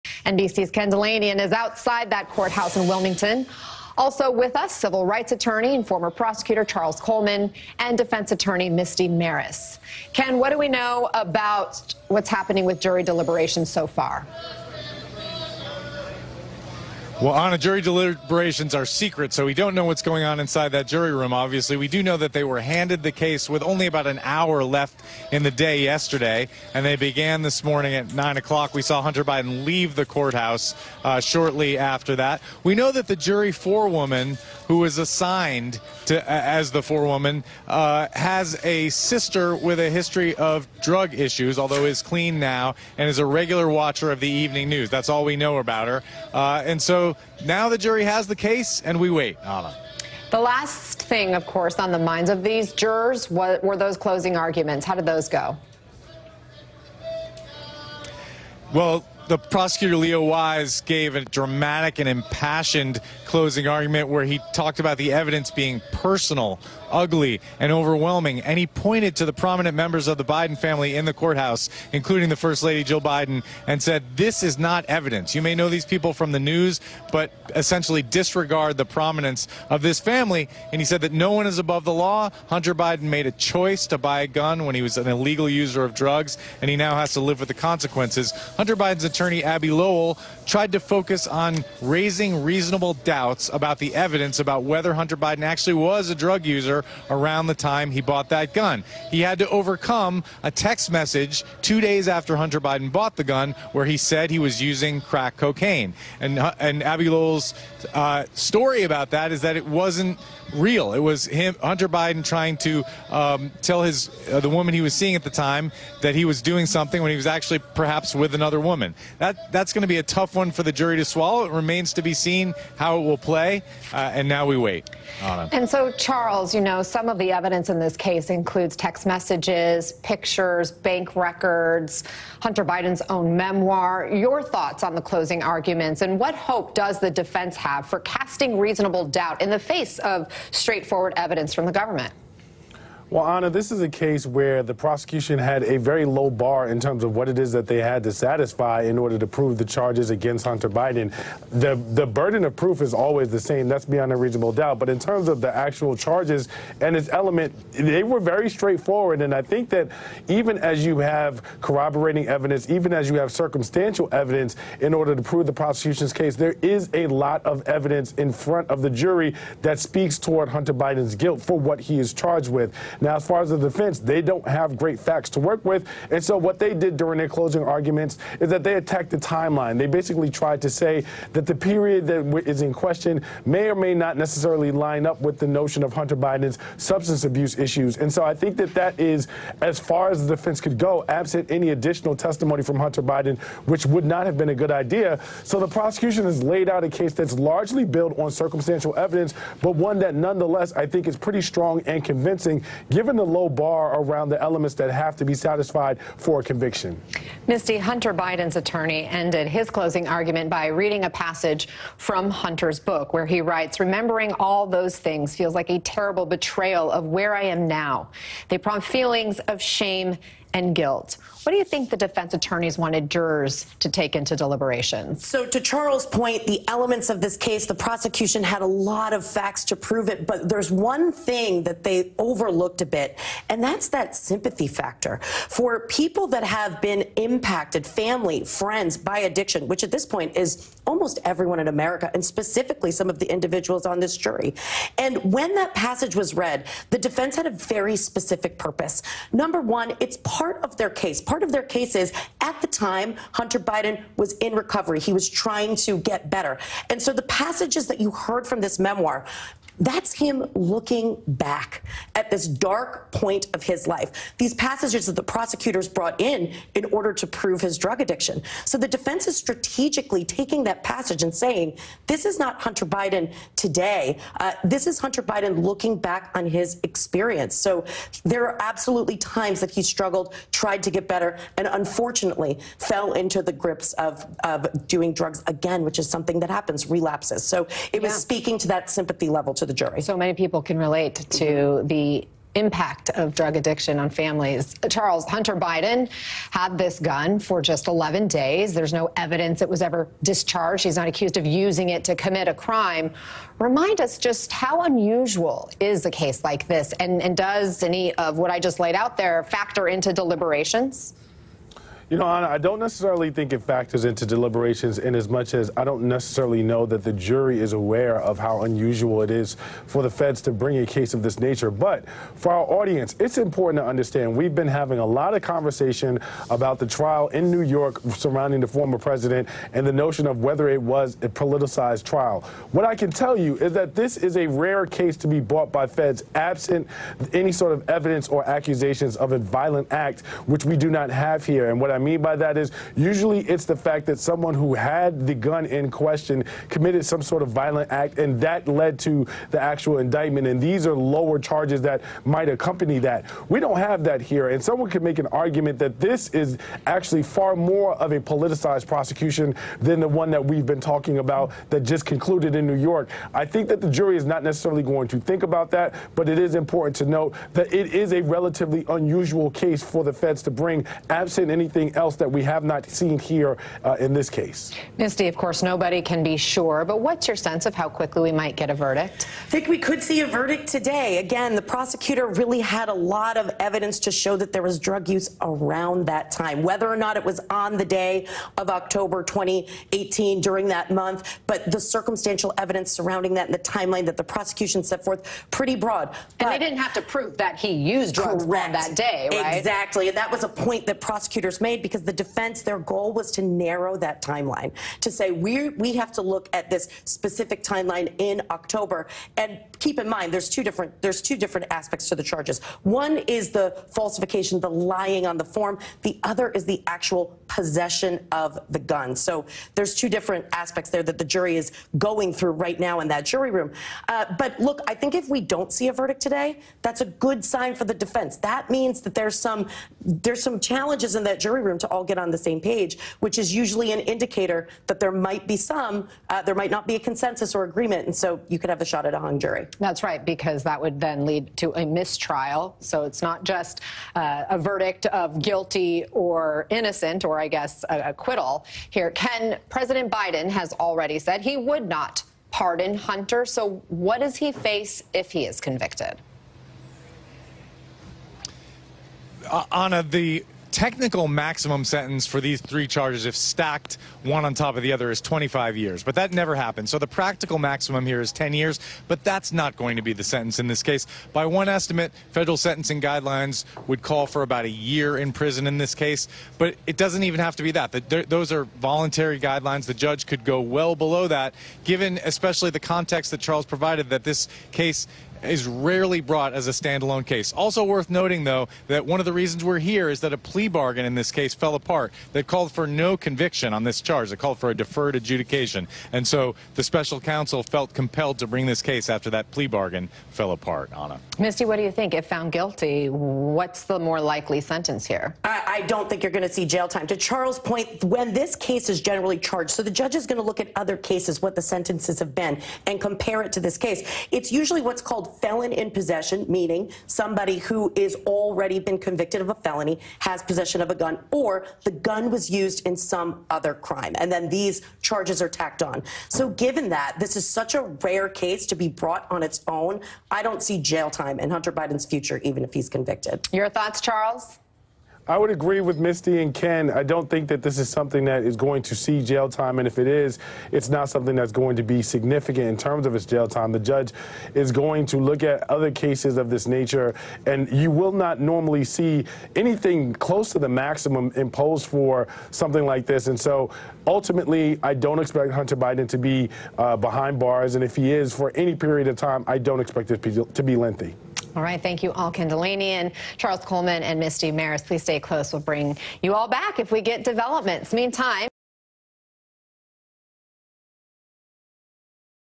Hunter Biden's Case: Legal Experts Weigh In on MSNBC | Jury Deliberations & Closing Arguments
In this segment from MSNBC, host Ana Cabrera discusses the ongoing jury deliberations in Hunter Biden's case with NBC's Ken Dilanian, civil rights attorney and former prosecutor Charles Coleman, and defense attorney Misty Marris. The panel explores the evidence presented, the impact of closing arguments, and potential outcomes for Hunter Biden. Misty Marris provides key insights into the defense's strategy and the role of sympathy in the jury's deliberations.